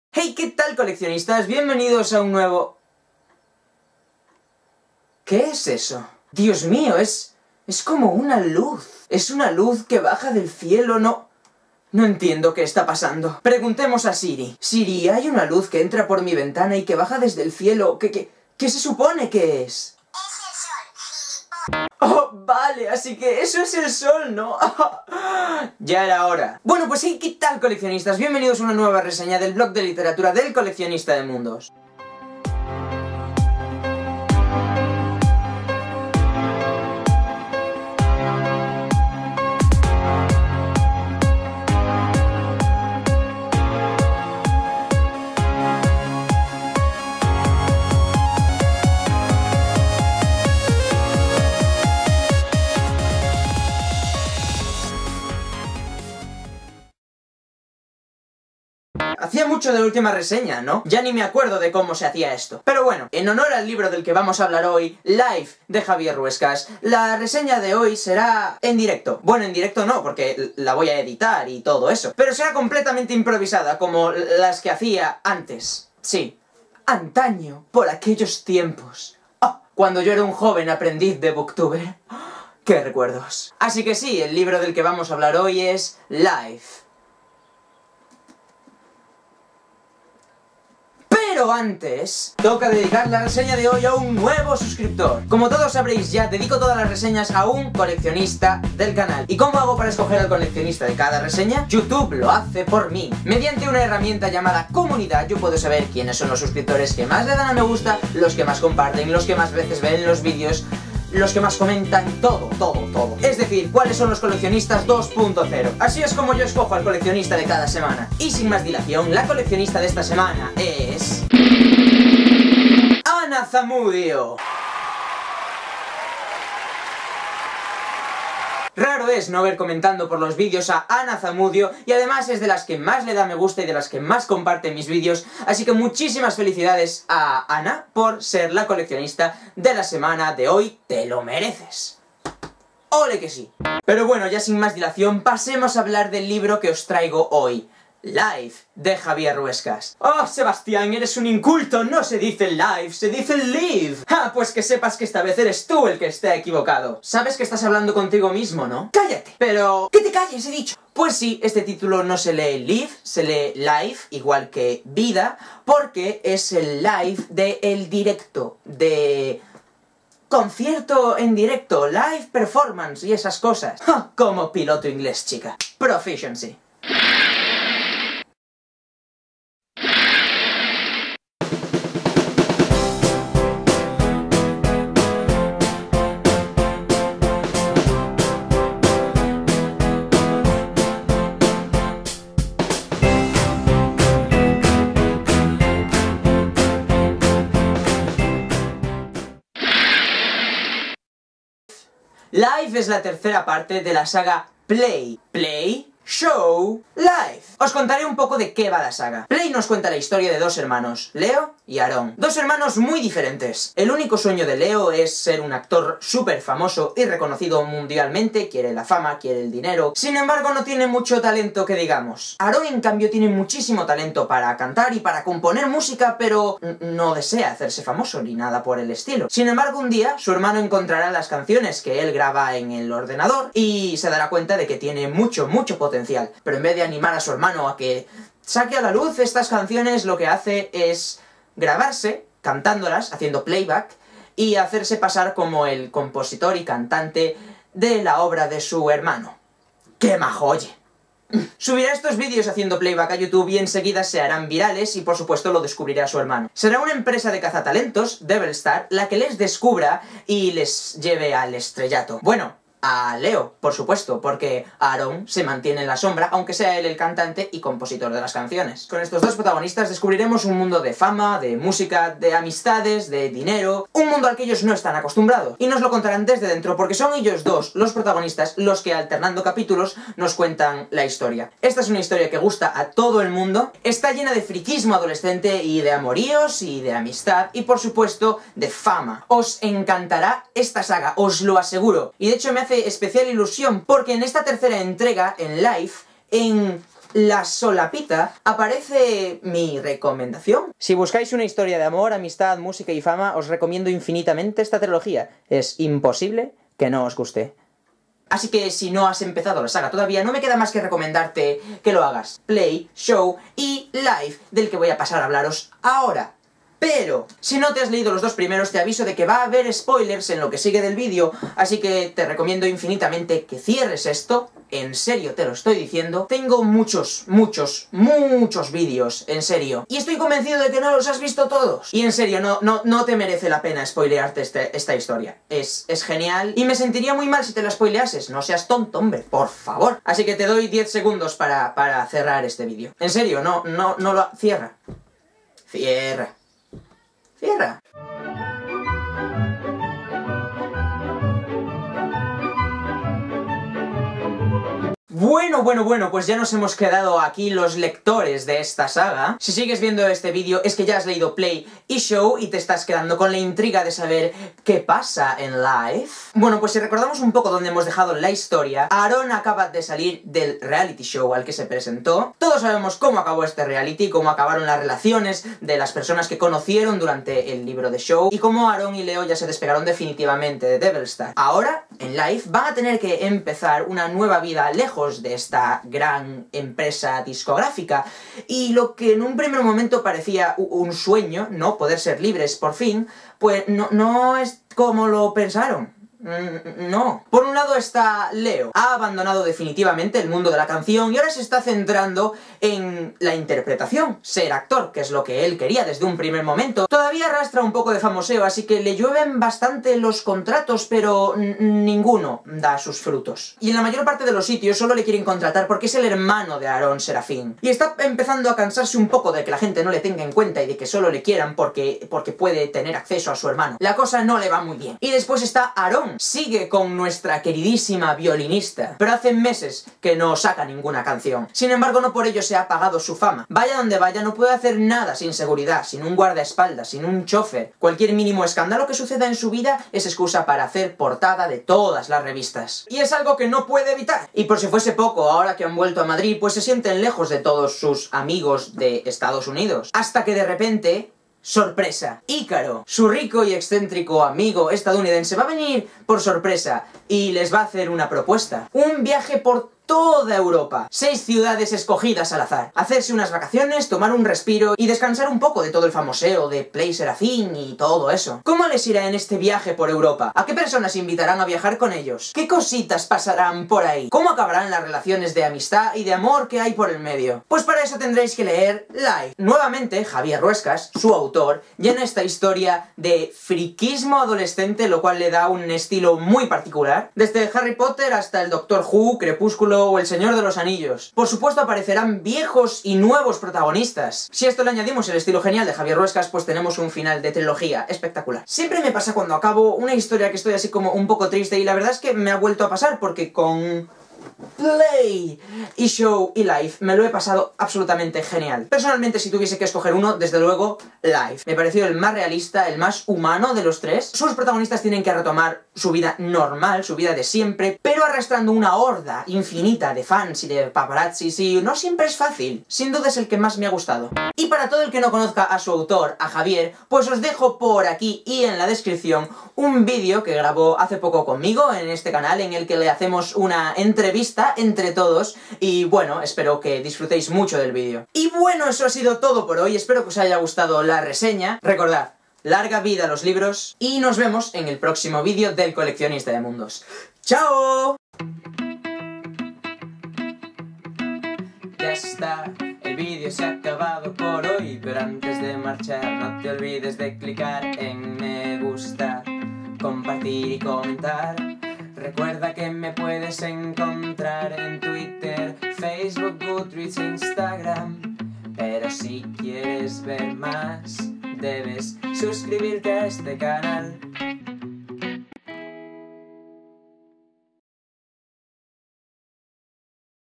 Reseña de "LIVE"
ＳＥＥ,　ＥＮＪＯＹ　ＡＮＤ　ＣＯＭＭＥＮＴ！

¡Hola! En esta nueva video-reseña "El coleccionista de Mundos" presenta el maravigenialísimo final de trilogía PLAY: "LIVE" de Javier Ruescas. Espero que encontréis interesantes mis vivencias literarias y, por supuesto, siempre sois bien recibidos

Música cedida por Liquid Muffin